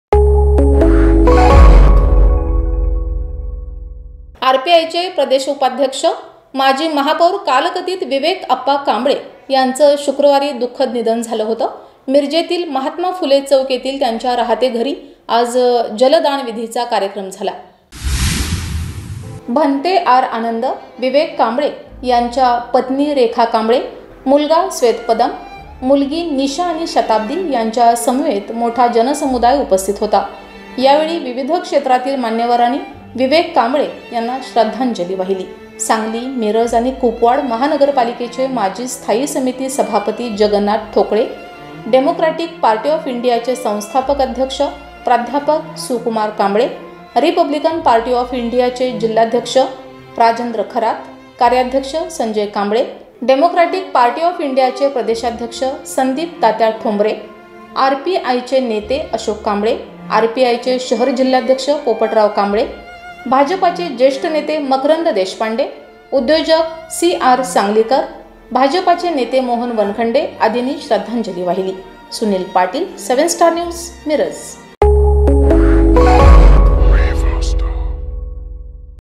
माजी महापौर कालकथित विवेक कांबळे यांना मान्यवरांकडून आदरांजली;
महात्मा फुले चौक येथे आज जलदानविधीचा कार्यक्रम संपन्न